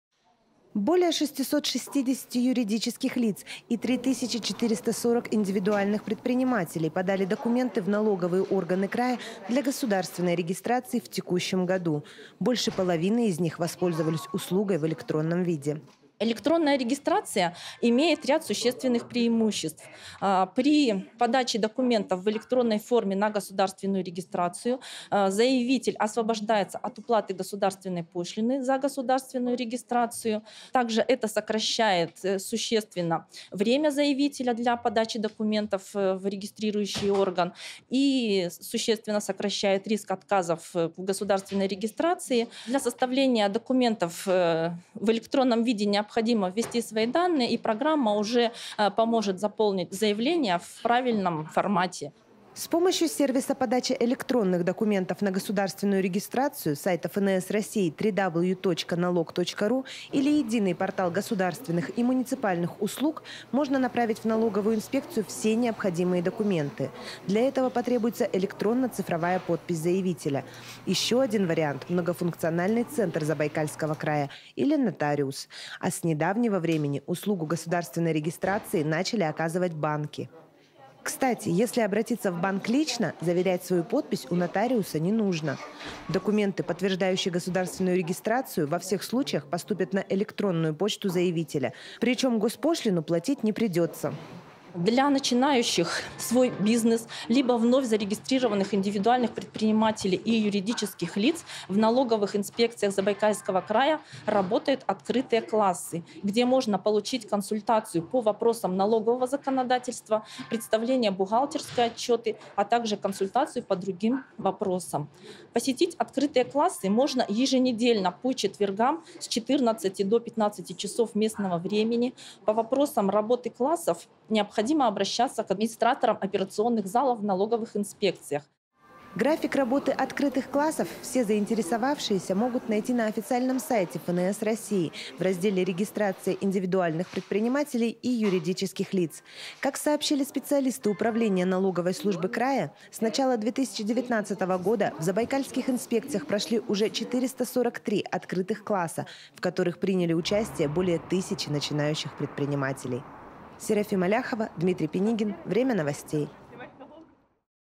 Электронная регистрация в УФНС Забайкалья
Около четырех тысяч юридических лиц или индивидуальных предпринимателей за год открыли бизнес в Забайкалье